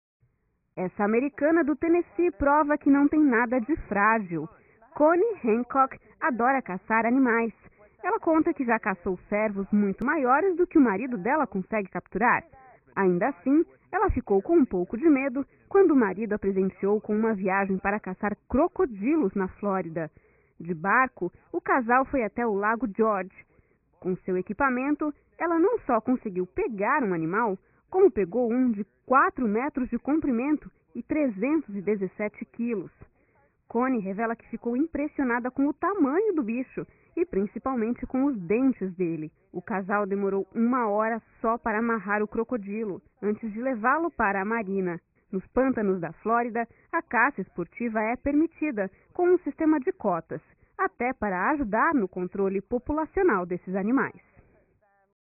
Mulher captura crocodilo de 4m e 317 kg nos EUA
Fã de caça, americana e o marido capturaram o bicho em lago da